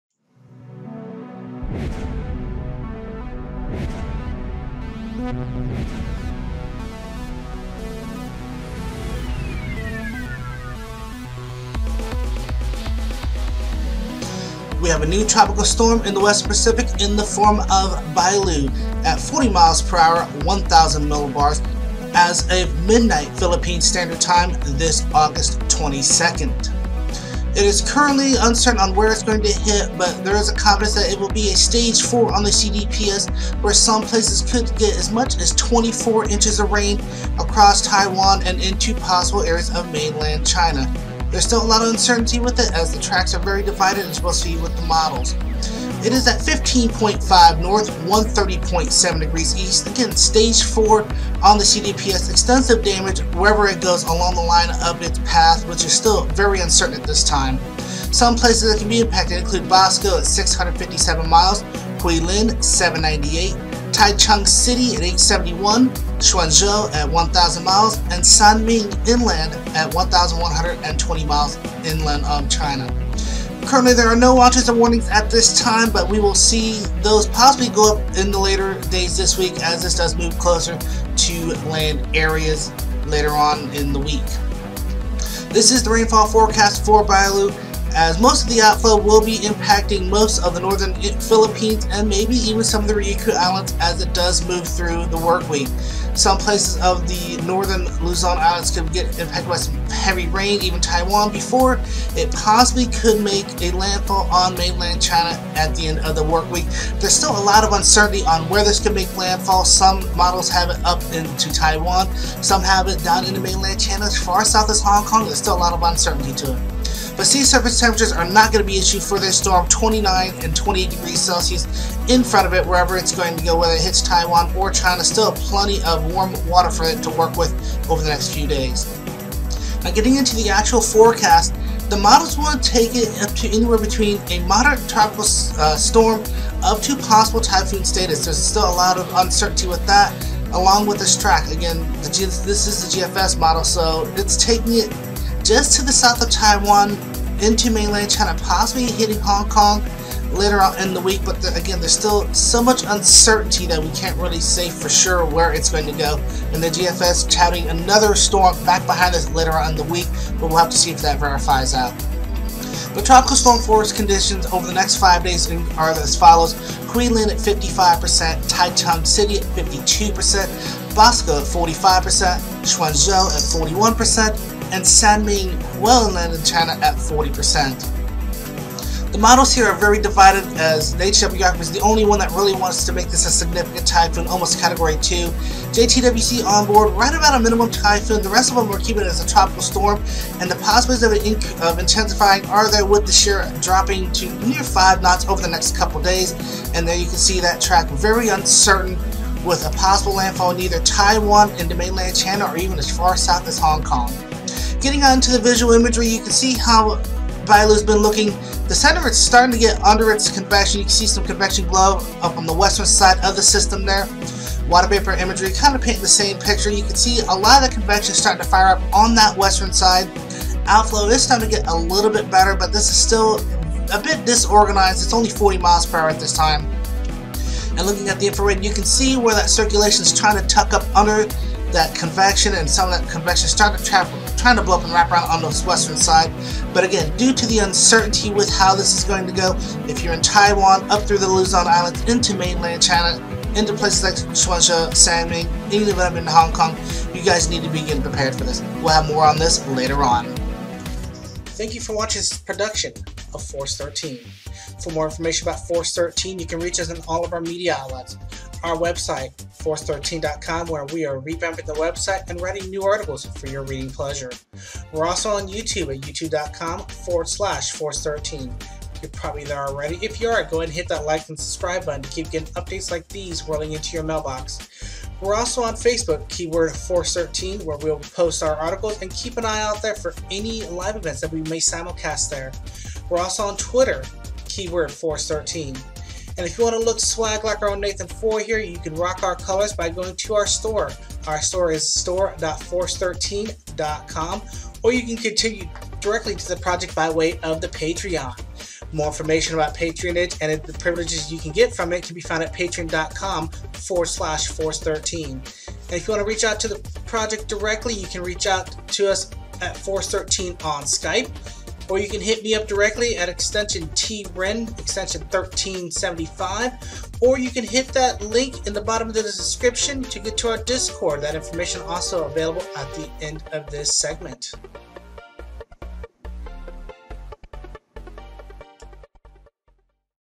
Tropical Storm Bailu Forms August 22, 2019 00:00 PHT
Tropical Storm Bailu has formed in the central western pacific and has a lot of uncertainty with its track and intensity, Models are divided on it just being a moderate tropical storm or if it will make a run at being a typhoon along with its track varying from Taiwan to as far south as Hong Kong.

Bailu is projected to be a stage 4 on the CDPS  regardless of where it impacts  dumping as much as 24 inches (610 millimeters) of rain across either  the northern Luzon Islands or into the mountains of Taiwan